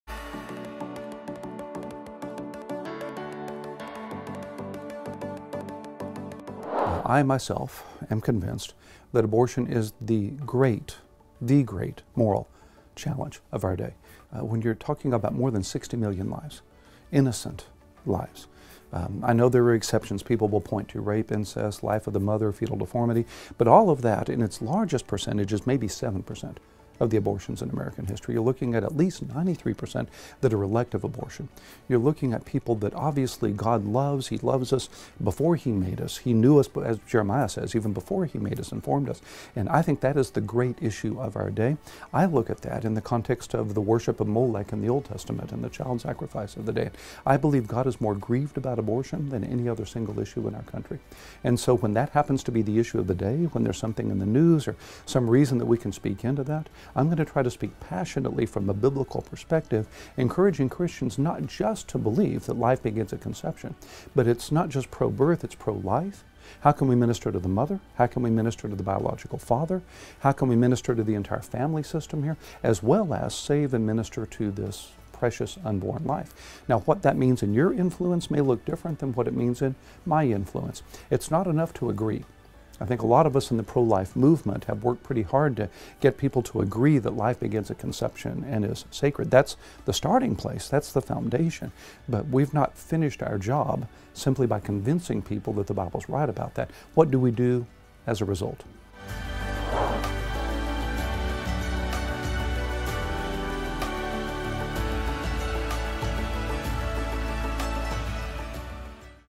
What is the greatest moral challenge of our day?
Megan sits down with Dr. Jim Denison and asks him, "What is the greatest moral challenge of our day?"